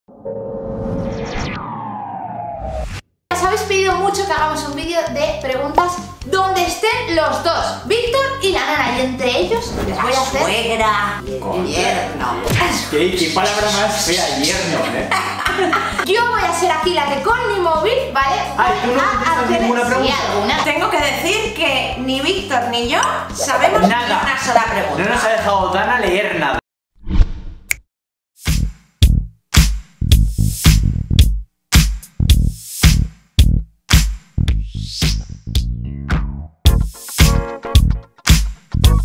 Danna, a Víctor: "Salvaría a Pedro antes que a ti" (PyR) | mtmad
¿Cómo lleváis la cuarentena? Danna y Víctor están en casa y han decidido pasar estos días con la madre de la influencer. Después de su viaje a París y someterse a un extraño retoque en la cara, juntos en familia, han decidido enfrentarse a las preguntas y situaciones más comprometidas y han despejado todas las dudas.

¿Qué piensa Víctor de que su suegra esté en casa estos días? ¿Cómo se llevan? ¿Y Danna? Si tuviera que elegir entre su chico o el padre de su hija, ¿con quién se quedaría? Además, la pareja y su madre han hablado de sexo y nos han desvelado sus posturas favoritas. ¡Descubre todas sus respuestas en el vídeo!